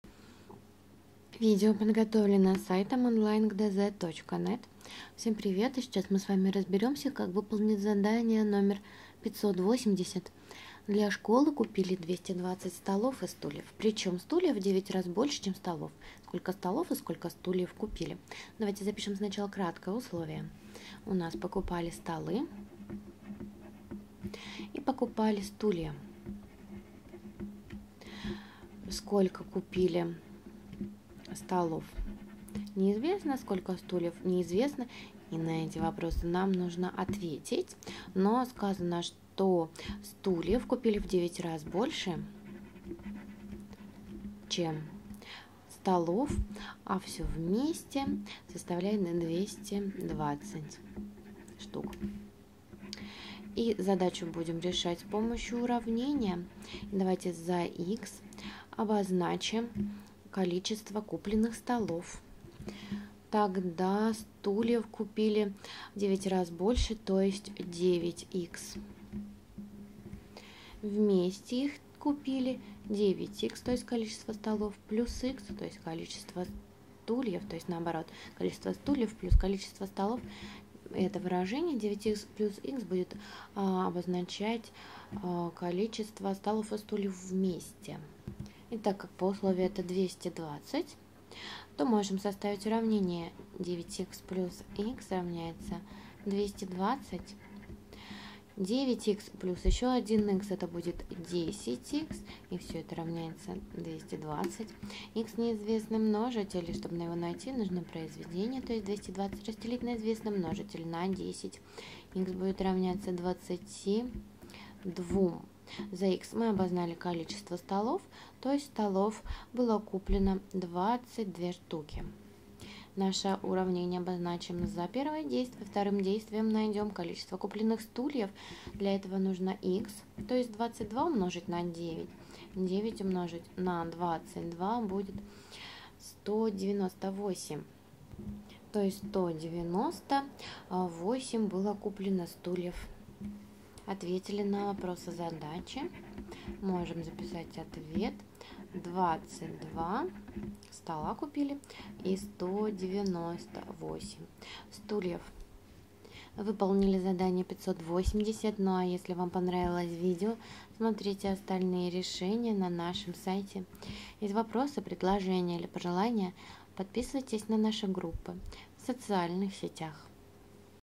Задание № 580 - Математика 5 класс (Виленкин, Жохов)
Задание 580 — текстовая задача, которую вам надо будет сделать с помощью уравнения. Равнение тут получится простое и его решение у вас займет не более минуты, главное разобраться, что тут потребуется за х принять — количество купленных столов или стульев, чтобы уравнение правильно составить. Если вы хотите без ошибок сделать это задание, посмотрите наше видео.